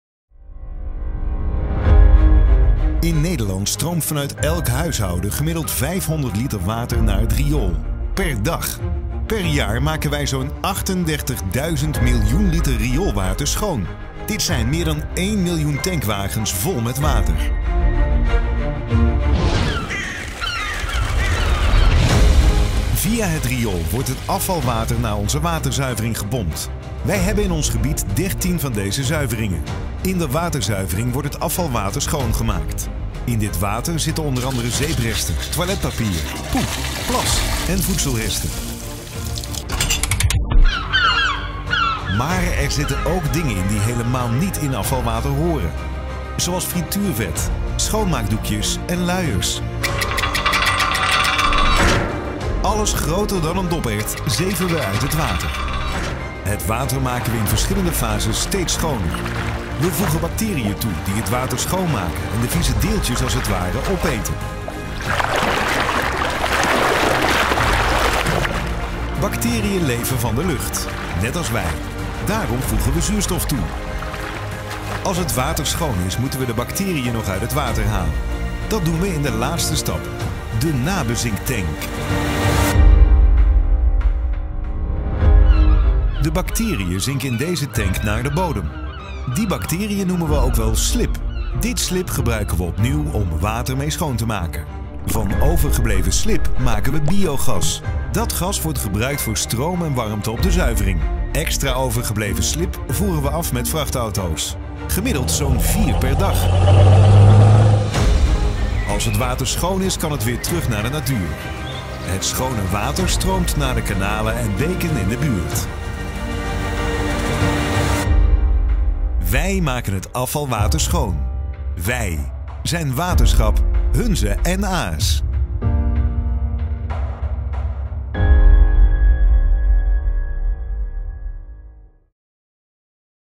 Schoonmaken afvalwater - waterschap
Waterschap Hunze en Aa's heeft 13 rioolwaterzuiveringsinstallaties (rwzi) om het rioolwater (afvalwater) schoon te maken. Hoe zie je in de film.